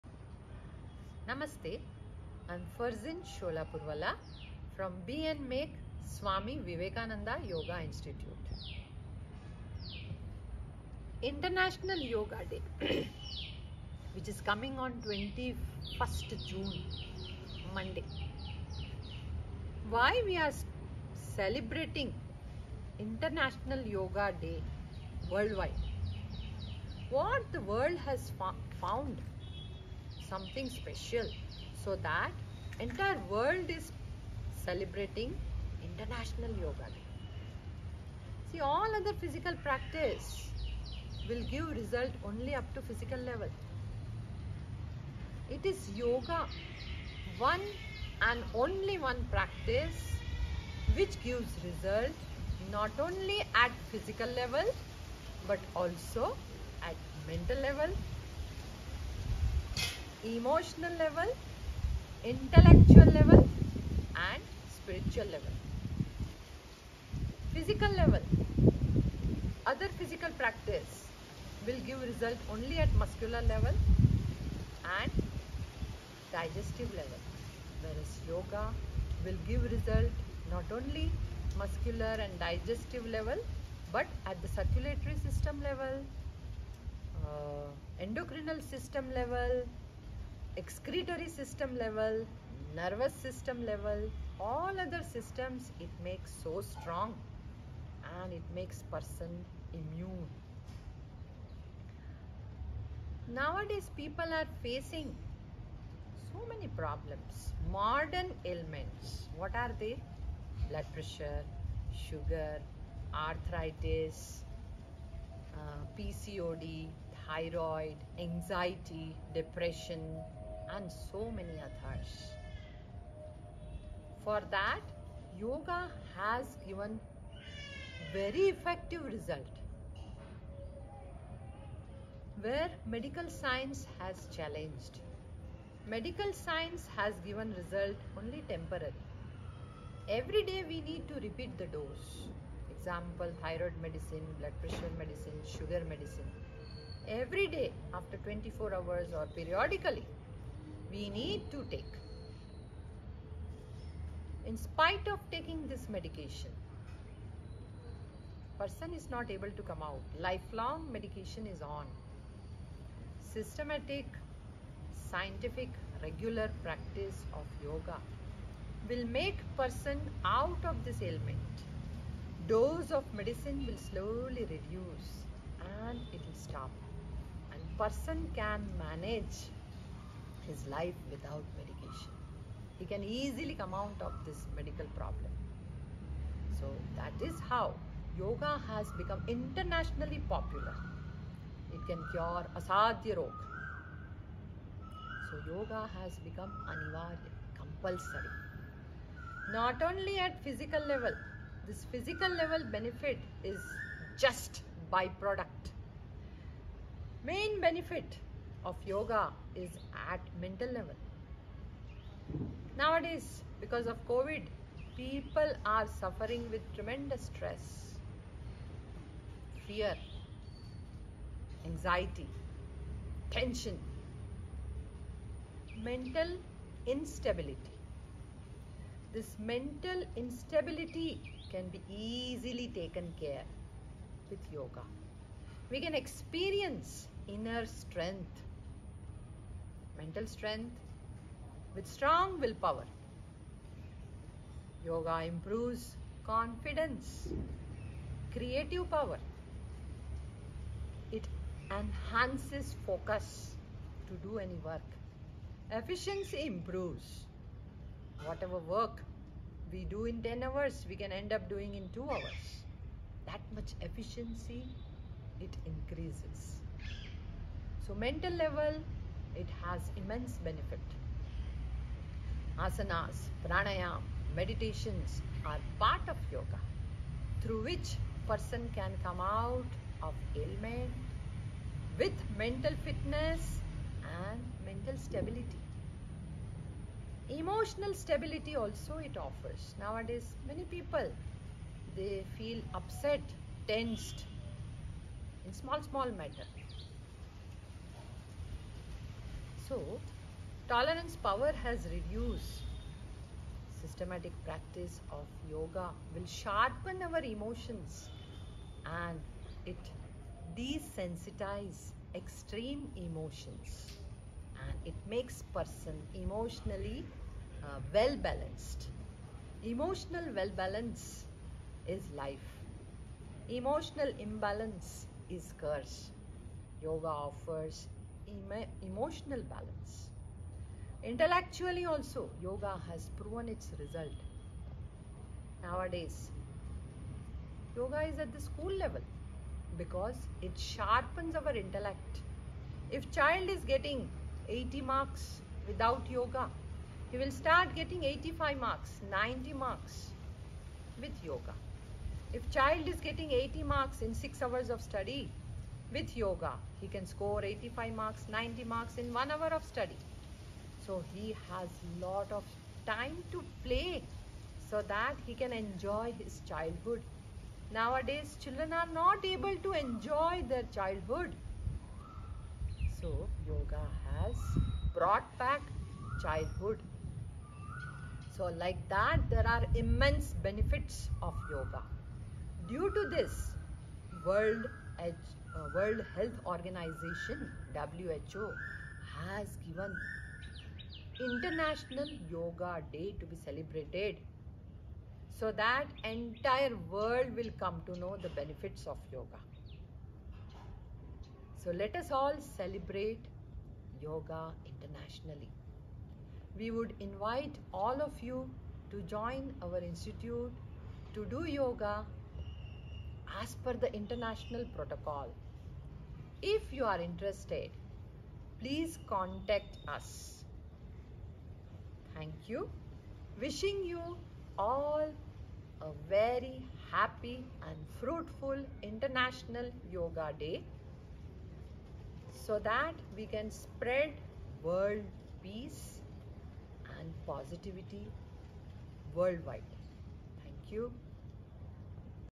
Is Yoga better than gym? Why is Yoga celebrated worldwide? | Be & Make Swami Vivekananda Yoga
Some people see it as a question…yoga or the gym — which is better? Is there really a question in there?
There are about a gazillion reasons a yoga class is better.
Yoga benefits the mind, body and spirit.
Yoga benefits your full body—externally and internally.
Yoga teaches acceptance, efficiency and accuracy.
Yoga is kinder to the body.
Yoga reduces stress, improves concentration, calms you down and reduces your aches and pains.

Please watch our beloved founder, Farzin Sholapurwala, as she delves into the fifth of a series of small and quick introductions to the beautiful art and science of life, known as Yoga.